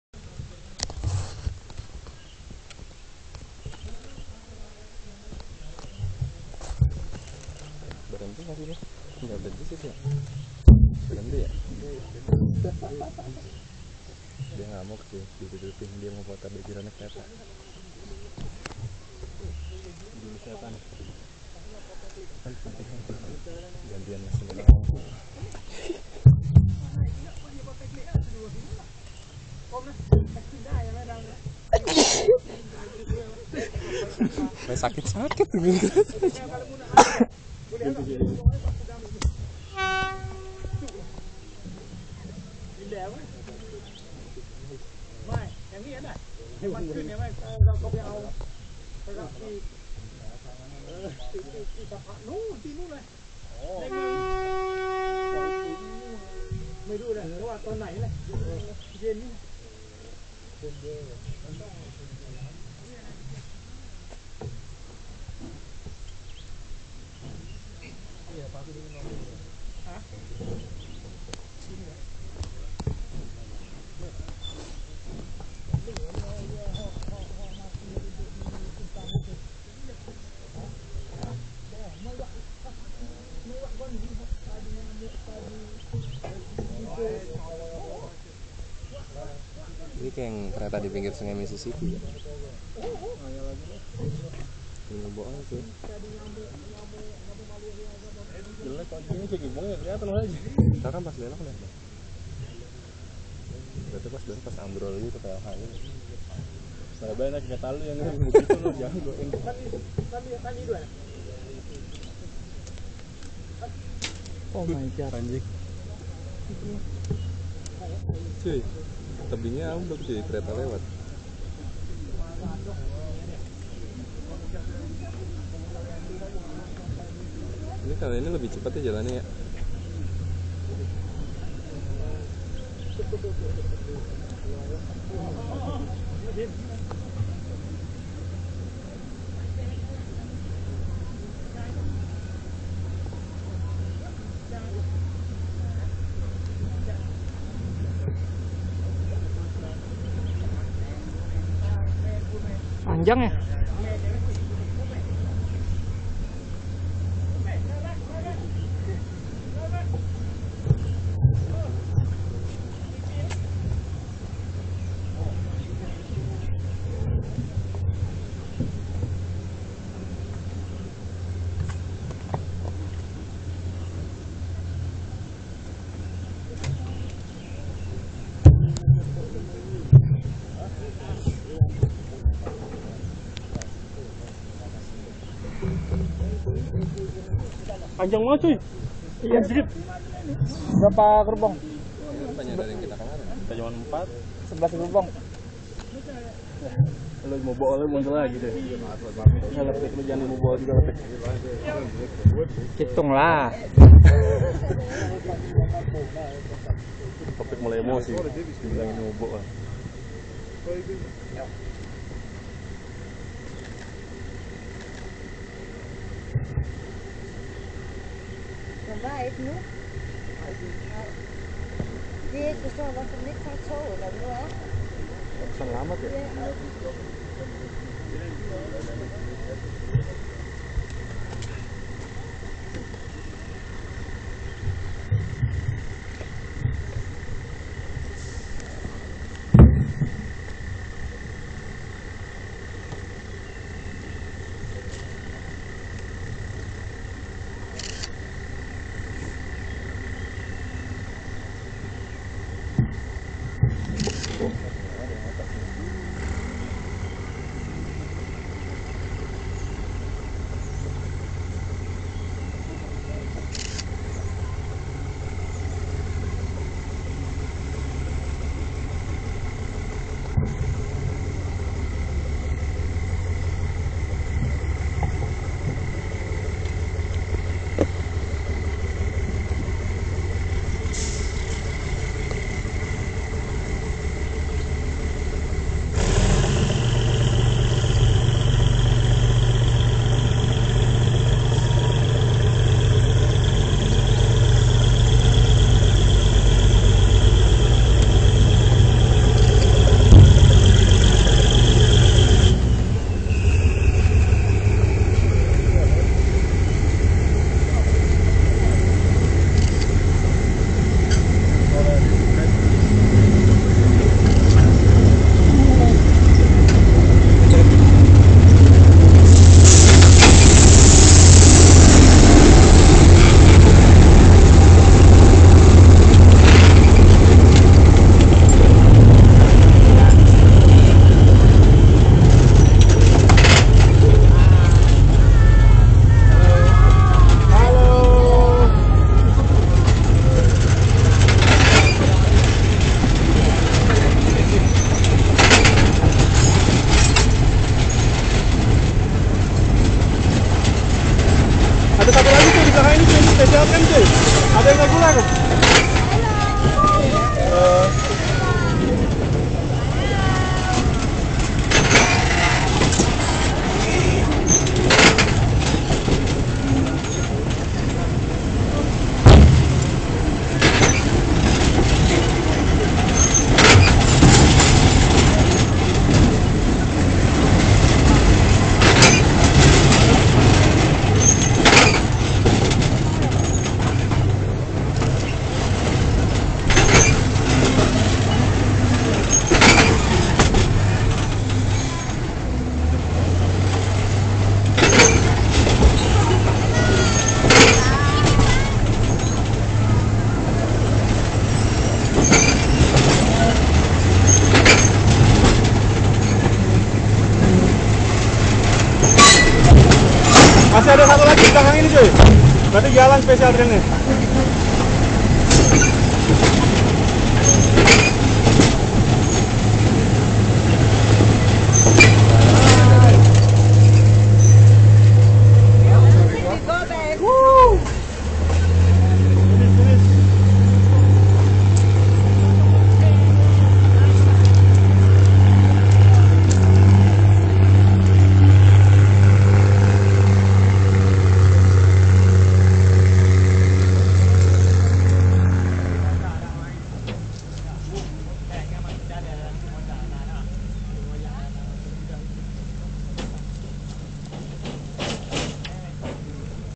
The Wooden Bridge of Death Railway
A long train on weekend crossing The Wooden Bridge, a part of The Death Railway line in Thailand ... video taking from the cliff side of the railway ...

Video shooting by Lenovo ThinkPad Tablet camera ... ^^